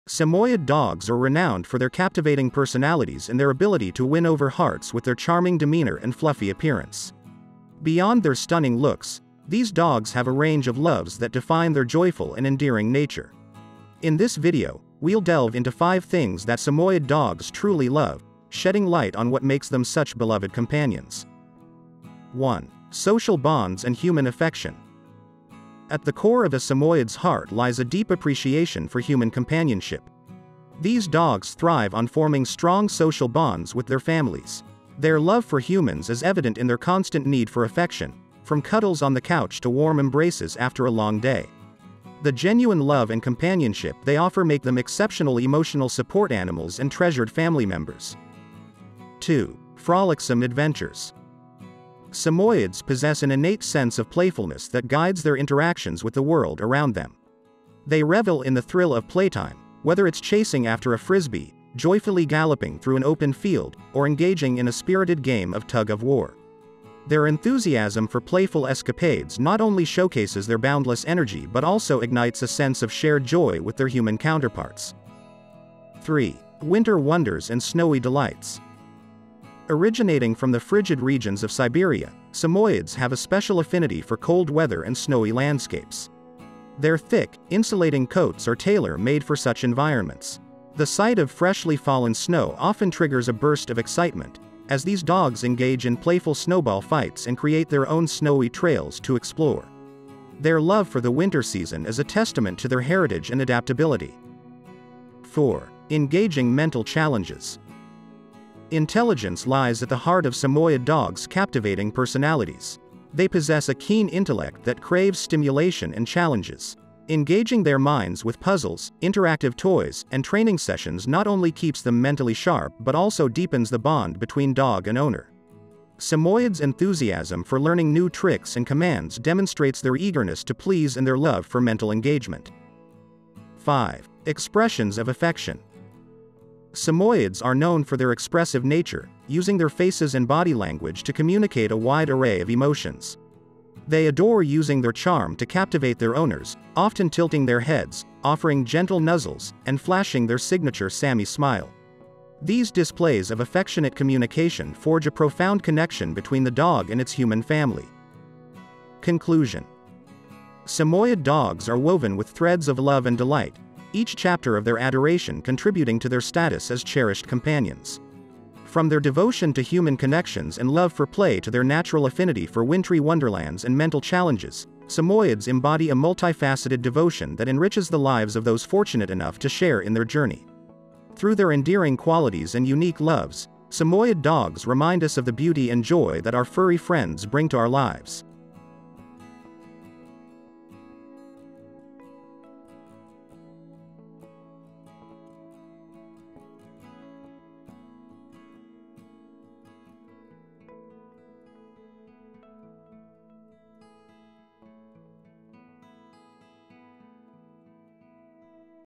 5 Things Samoyed Dogs Love the Most
Samoyed dogs are renowned for their captivating personalities and their ability to win over hearts with their charming demeanor and fluffy appearance.
Beyond their stunning looks, these dogs have a range of loves that define their joyful and endearing nature. In this exploration, we'll delve into five things that Samoyed dogs truly love, shedding light on what makes them such beloved companions.